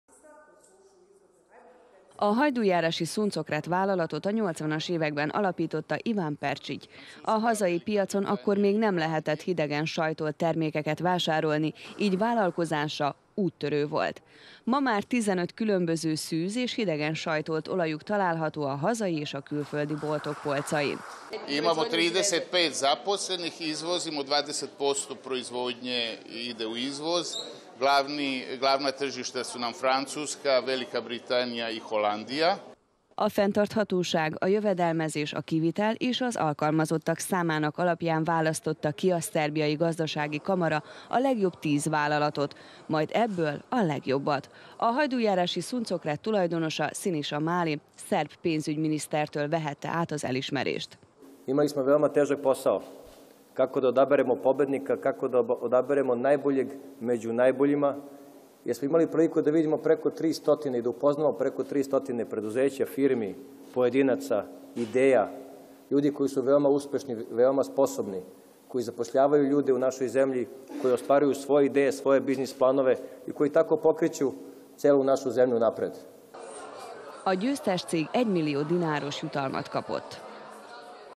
2018.07.12. - Hajdújárási vállalat az év Blic-vállalkozója
Szűz és hidegen sajtolt növényi olajok és vajak, ecetek, mézes-magvas energia kockák. Ezekről a termékekről nevezetes a hajdújárási Suncokret üzem. Tulajdonosát kiáltotta ki a legsikeresebbnek a Blic.